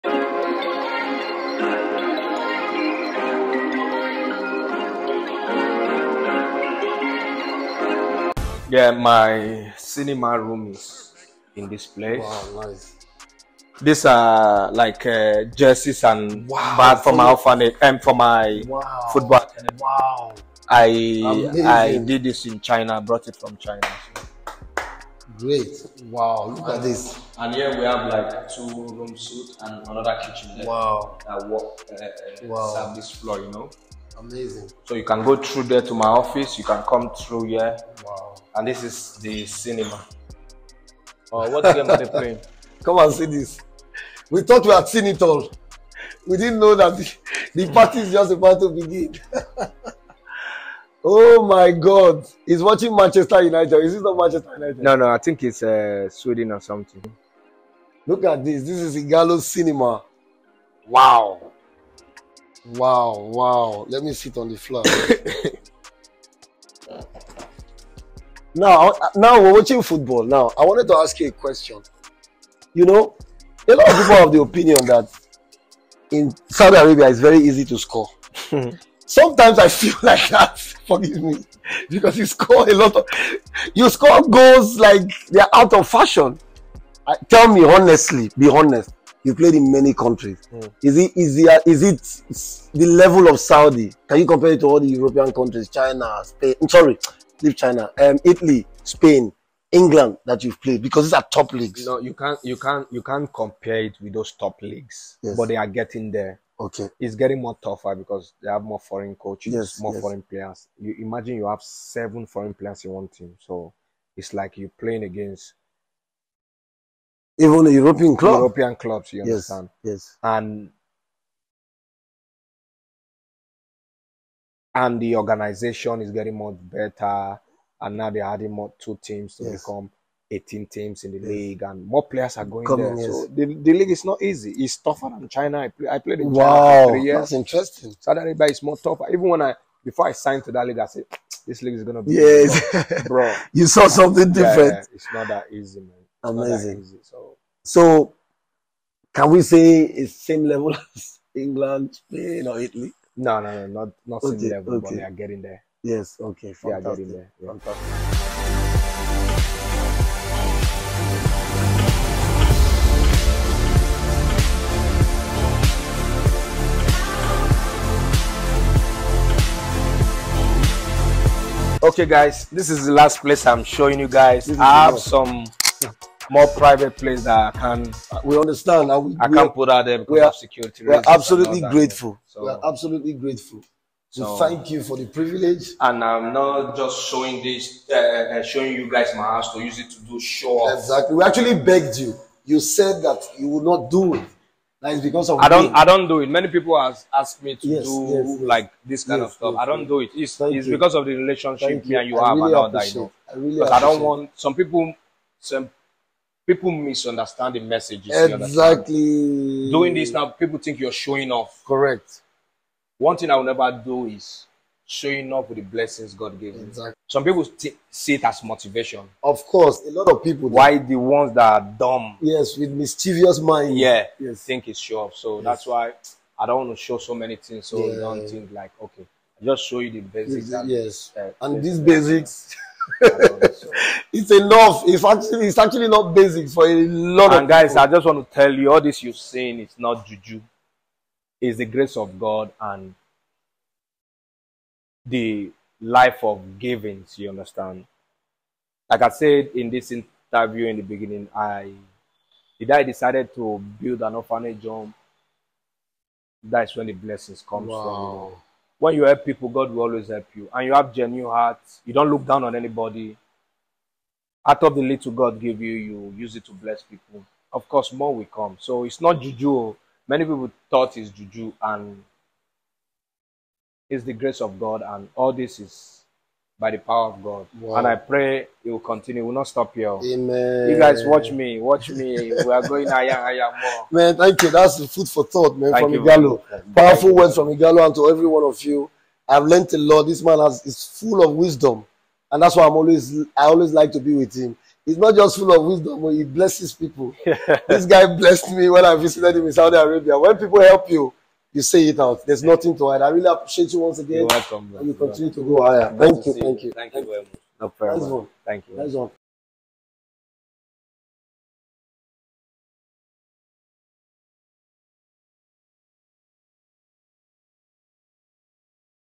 At Home With Ighalo|Part 4.
On the concluding part, we spent time inside his home cinema while we discussed the the quality of football in Saydi Arabia.